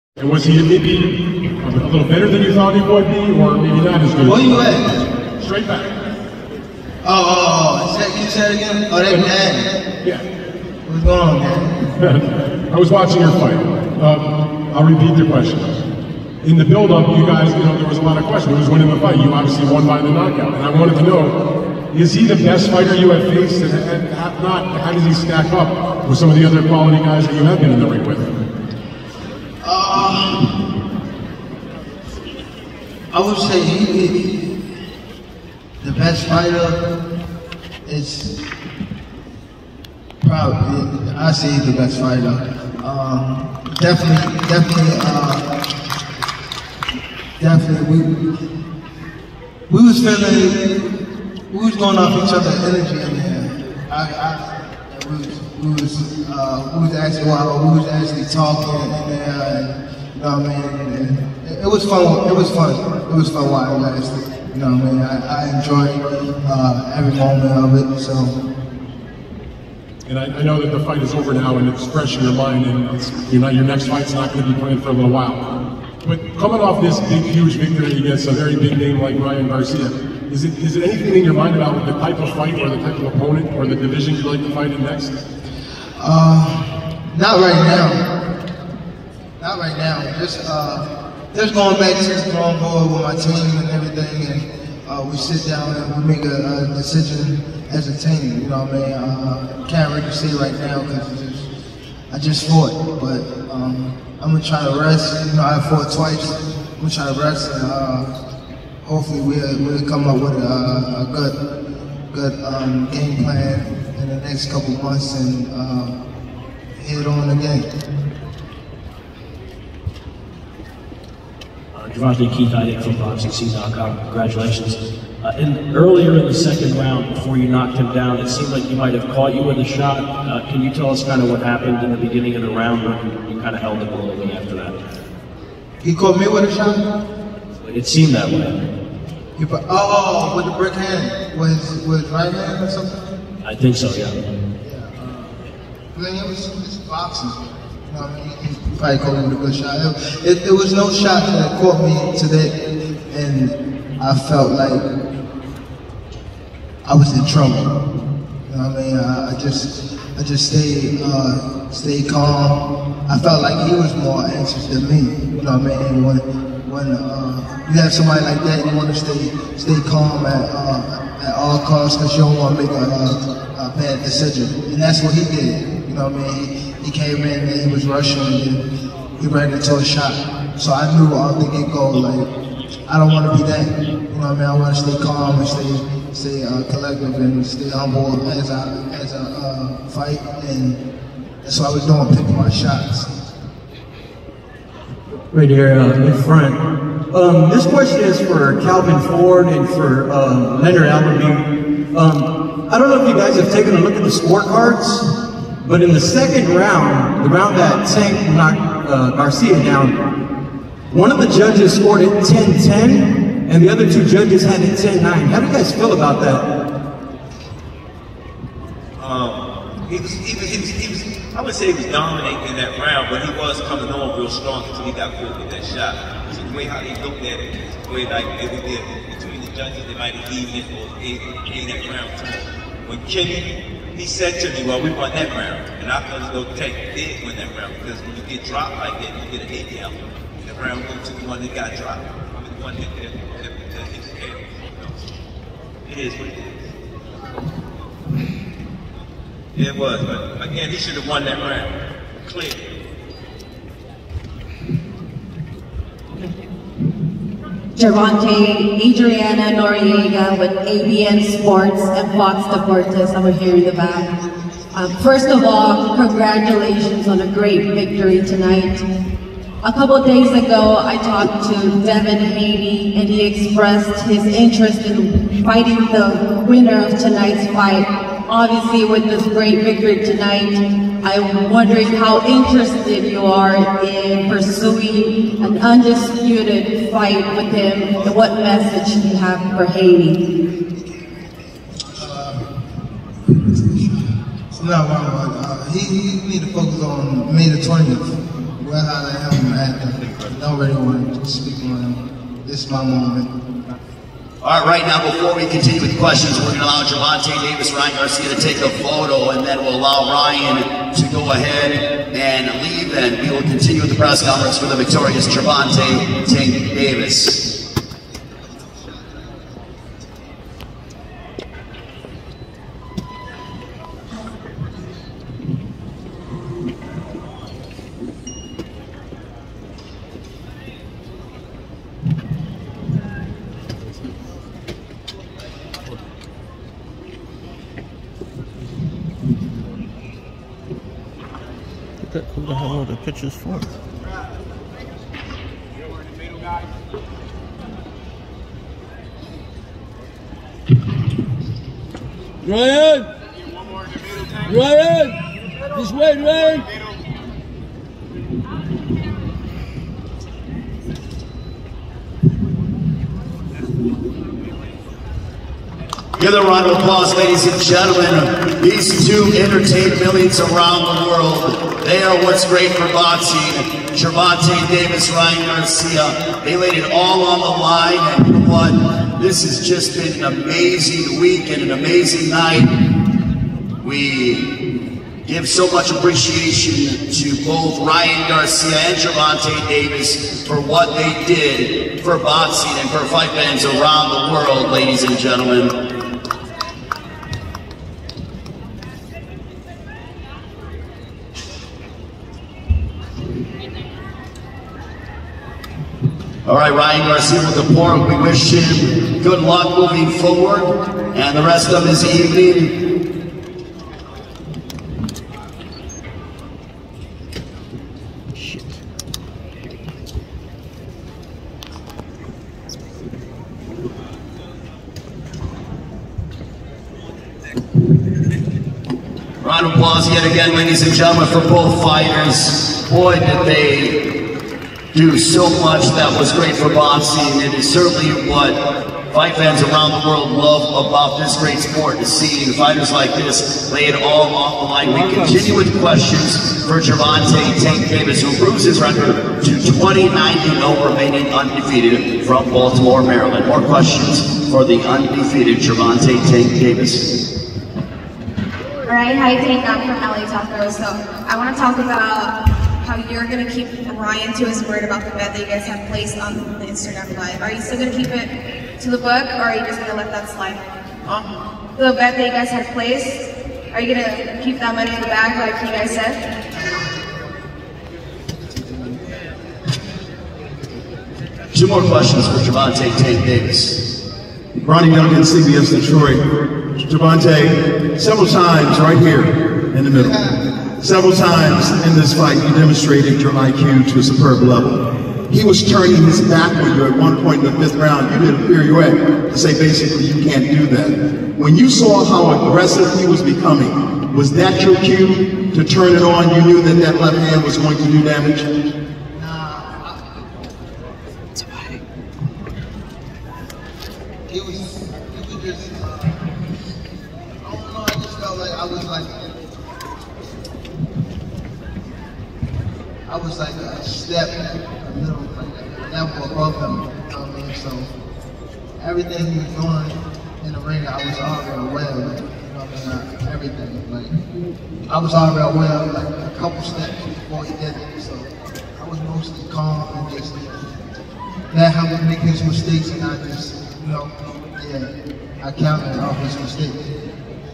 Gervonta Davis talks Devin Haney, poses for photos with Ryan Garcia after win
Undefeated three-division champion Gervonta Davis discusses Devin Haney after being told that the champion wanted the winner of is matchup against Ryan Garcia, then poses for photos with the fighter he just defeated in the ring after the fight at T-Mobile Arena in Las Vegas, Nevada